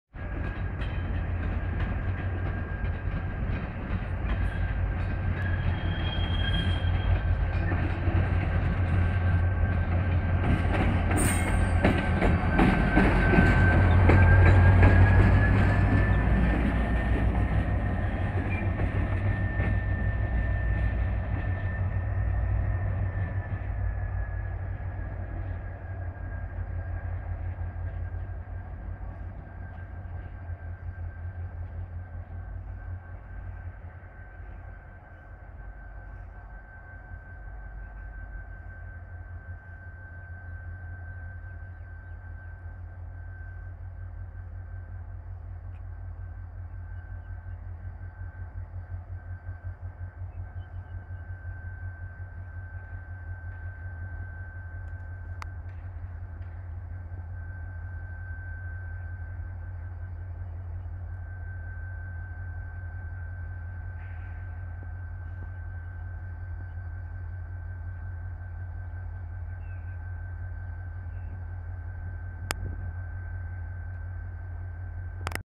GT22 A918 ( ex LSM ) Finalizando maniobras en Plaza C.
En este video podran ver a la locomotora GT22 A918 despues de haber dejado a ordenes a la GT22 9085 de Ferrobaires en el desvio ir para quedar a ordenes en Plaza Constitucion de la Linea Roca